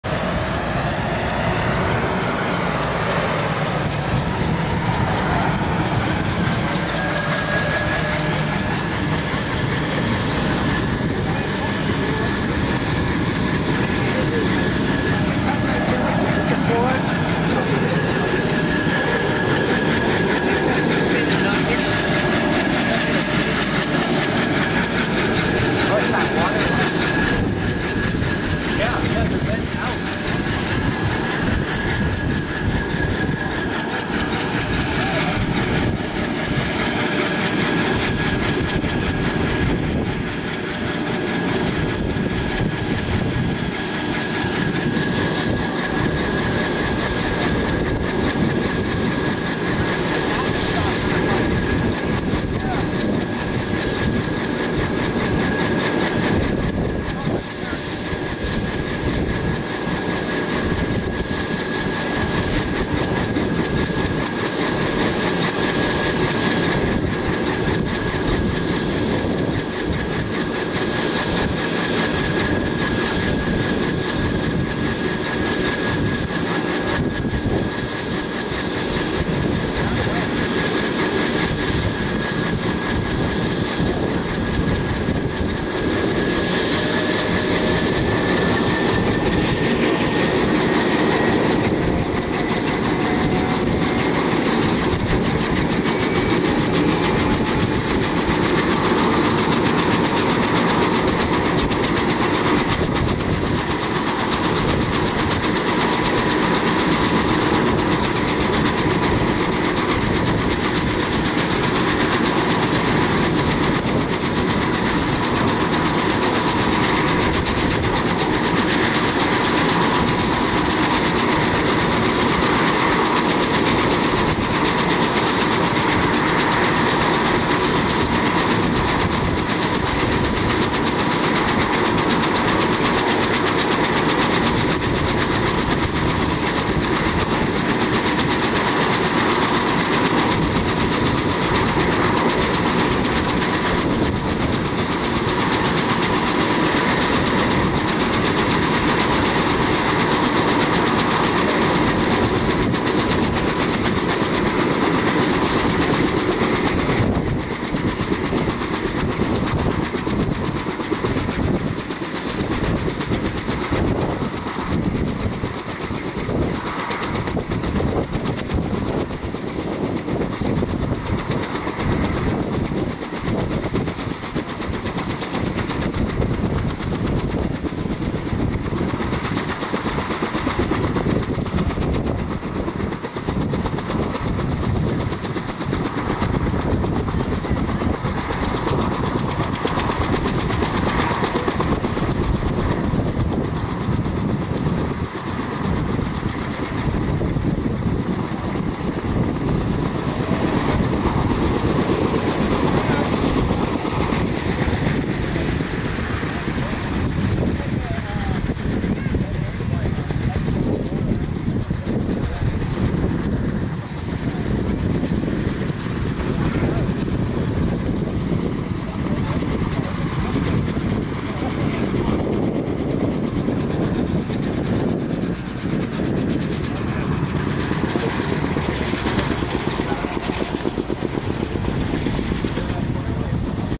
Olympic Airshow—Kaman HH-43 Huskie starting and takeoff
I was testing out a Sony A77M2 body at this airshow. This video clip is straight out of the camera. The lens is a Minolta 70-200 f/2.8 SSM. It definitely needs some post processing, but I guess that's normal. What's weird is the little "buzzing" vibration you see from time to time, like when I zoom in on the rotor masts. I'll do some more experimenting before deciding whether or not I'll keep the body.